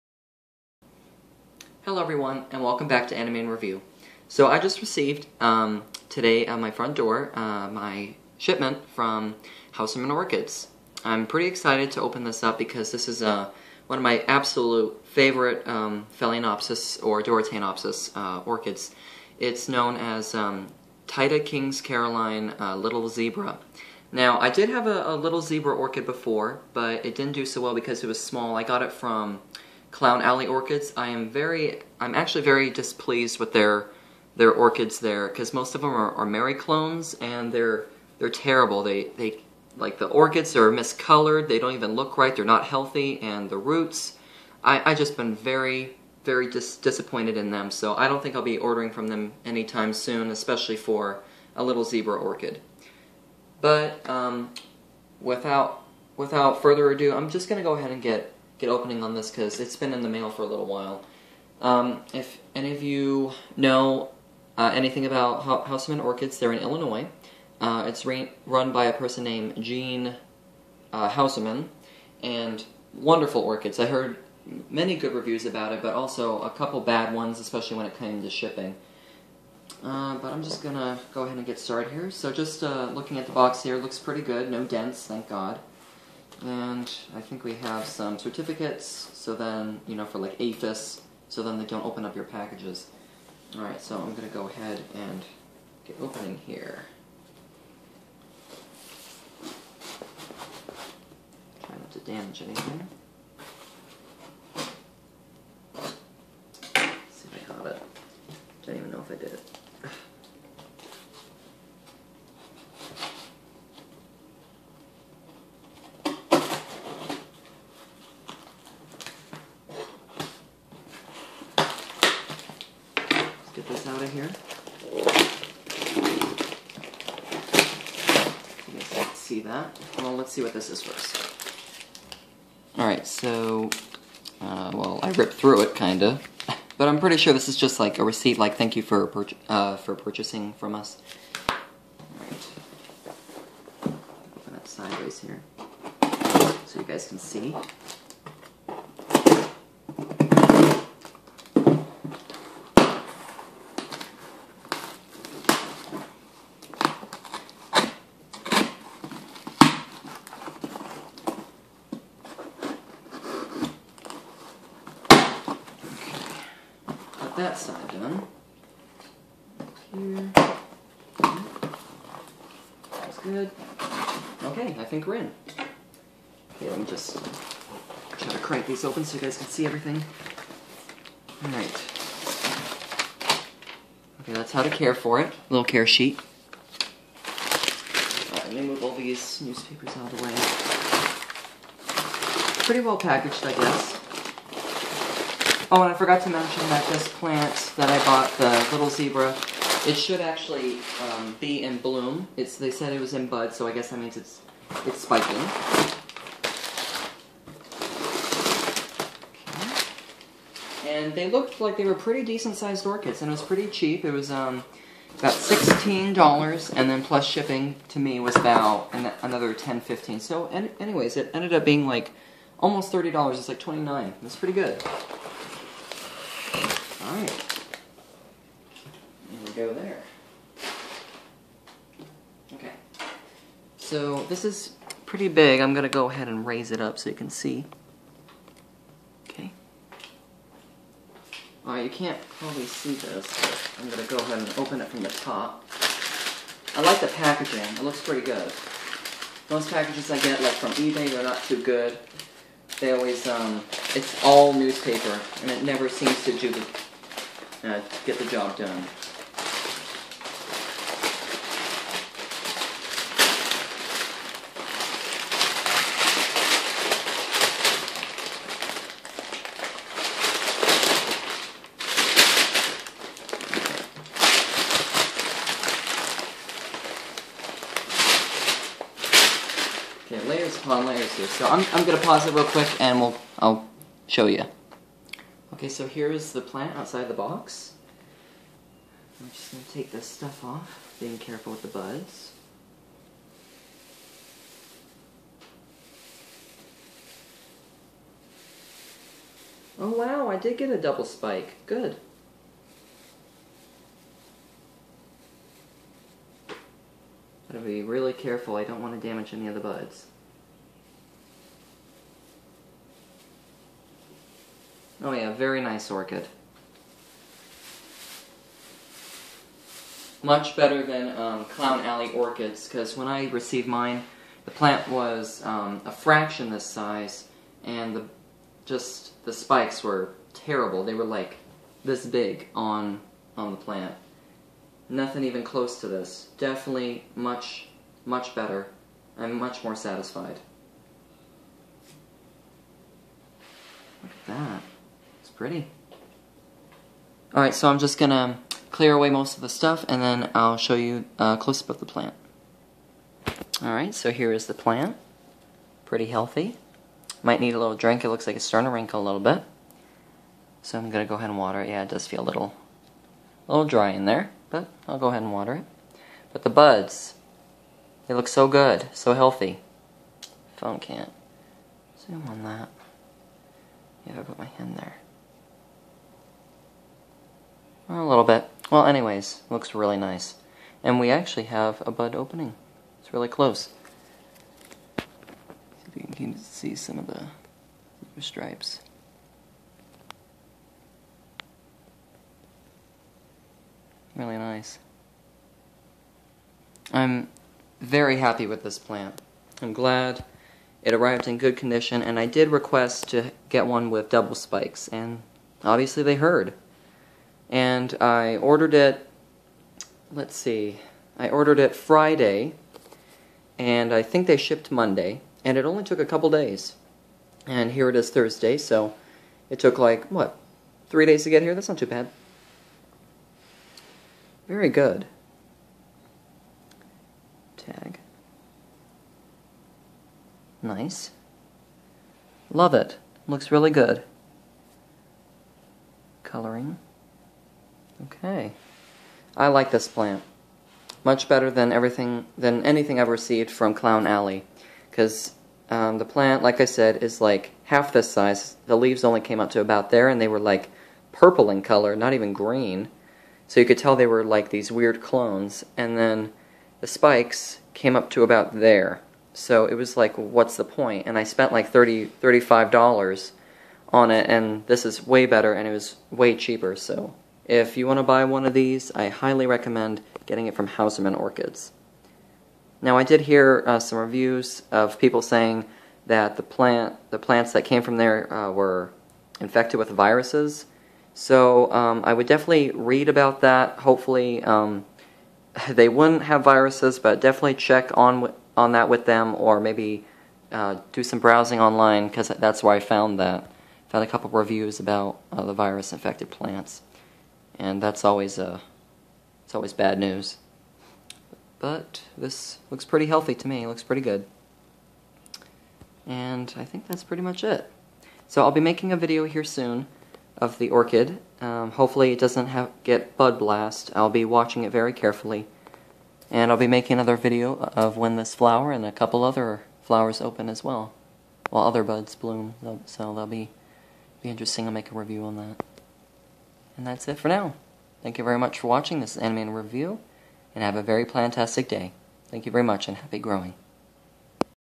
Phalaenopsis Taida "Little Zebra" Orchid
Hey guys, I am so happy that I was finally able to get my hands on a mature Phal. Taida "little zebra" orchid. I purchased this orchid from Hausermanns orchids for around $30 with shipping included. I arrived in bloom and has already finished flowering, I shall make an updated video real soon. Thanks again!!!